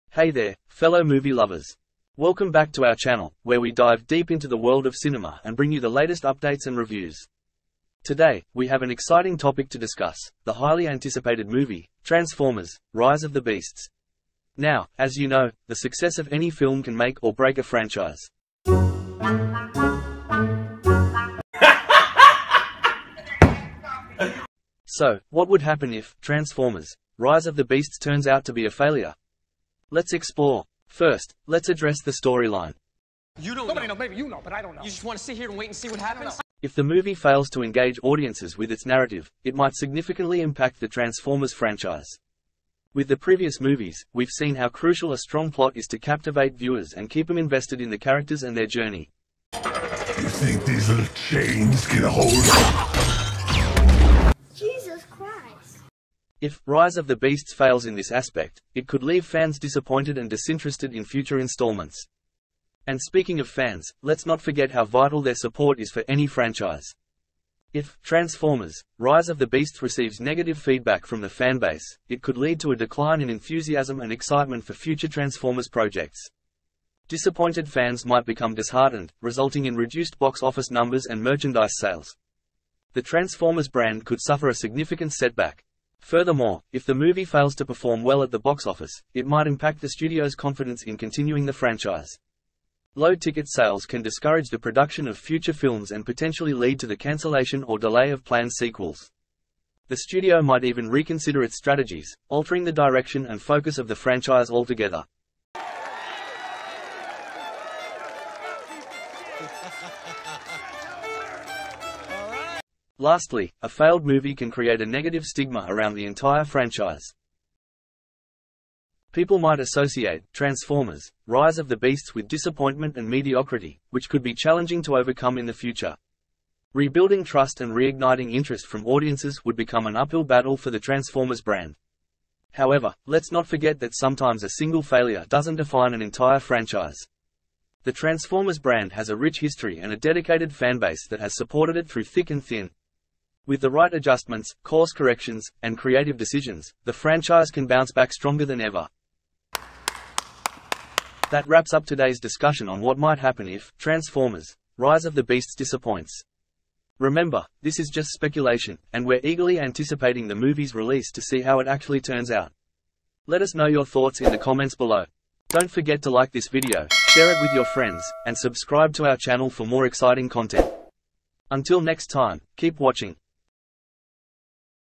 Transformers: Rise of the Beasts' Box Office Nightmare? What's at Stake for the Franchise!
Exploring the consequences of "Transformers: Rise of the Beasts" flopping at the box office. What's at stake for the franchise? Join us as we analyze the potential impact and future implications. Find out what's in store for the Transformers universe if this film fails to meet expectations. Don't miss this insightful discussion!